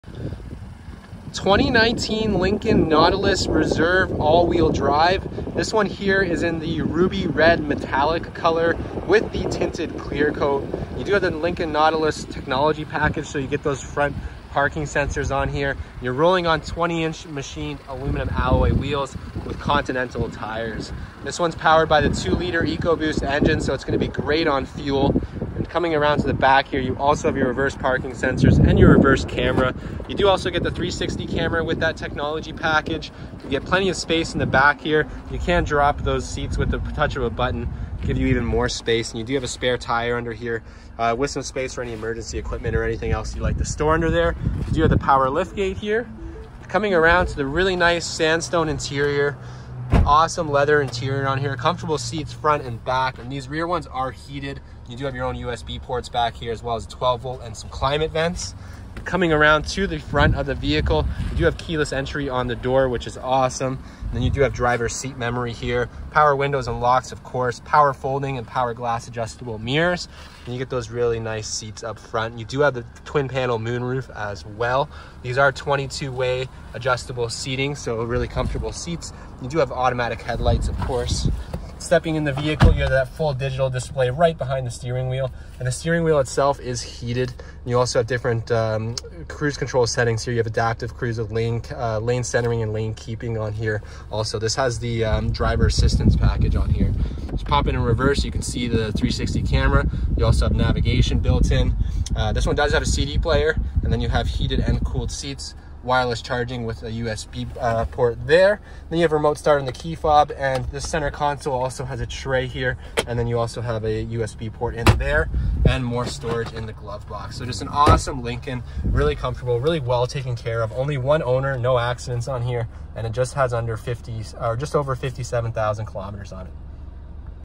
2019 Lincoln Nautilus Reserve AWD w/ Technology Package in Ruby Red Metallic Walk-Around
One-Owner|No Accidents|Very Clean|AWD Reserve|Ruby Red Metallic Tinted Clearcoat|2.0L EcoBoost|20" Aluminum Alloy Wheels|Panoramic Sunroof|Technology Package w/ 360 Camera|Wireless Charging|Front Parking Sensors|Heated and Cooled Power Front Seats|22-Way Power Front Driver Seat|Heated Rear Seats|Heated Steering Wheel|Driver Assistance Package|Remote Start|Cappiuccino Leather Interior|Navigation